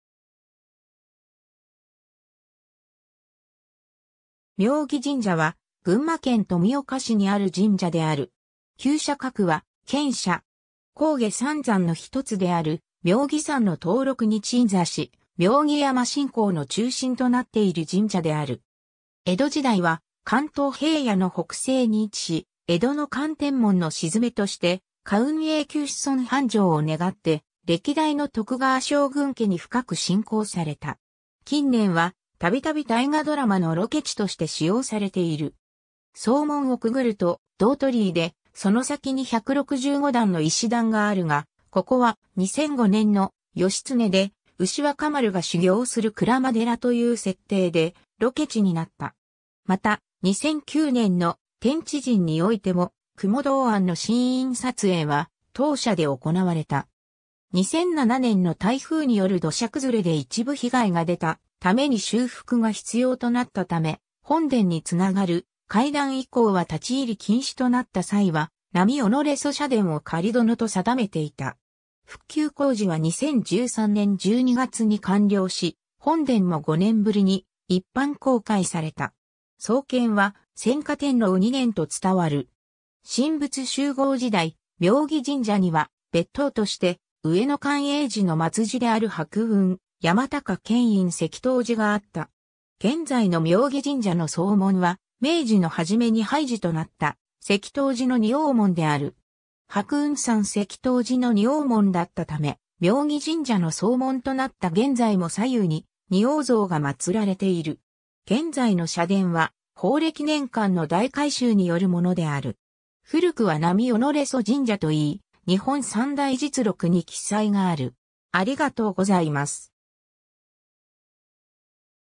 妙義神社
重要文化財_(建造物)
妙義神社（みょうぎじんじゃ）は、群馬県富岡市にある神社である。
 上毛三山の一つである妙義山の東麓に鎮座し、妙義山信仰の中心となっている神社である。
江戸時代は関東平野の北西に位置し、江戸の乾（戌亥）天門の鎮めとして、家運永久子孫繁昌を願って歴代の徳川将軍家に深く信仰された。
 近年は、たびたび大河ドラマのロケ地として使用されている。
総門をくぐると銅鳥居で、その先に165段の石段があるが、ここは、2005年の「義経」で、牛若丸が修行する鞍馬寺という設定でロケ地になった。
また、2009年の「天地人」においても、雲洞庵のシーン撮影は、当社で行われた。
 2007年の台風による土砂崩れで一部被害が出たために修復が必要となったため、本殿に繋がる階段以降は立ち入り禁止となった際は、波己曽社殿を仮殿と定めていた。
復旧工事は2013年12月に完了し、本殿も5年ぶりに一般公開された。
 創建は、宣化天皇2年（537年）と伝わる。
 神仏習合時代、妙義神社には別当（神社を管理する寺）として上野寛永寺の末寺である白雲山高顕院石塔寺があった。
現在の妙義神社の総門は、明治の初めに廃寺となった石塔寺の仁王門である。
白雲山石塔寺の仁王門だったため、妙義神社の総門となった現在も左右に仁王像が祀られている。
 現在の社殿は、宝暦年間（1751年 - 1764年）の大改修によるものである。
古くは波己曽（はこそ）神社といい、『日本三代実録』に記載がある。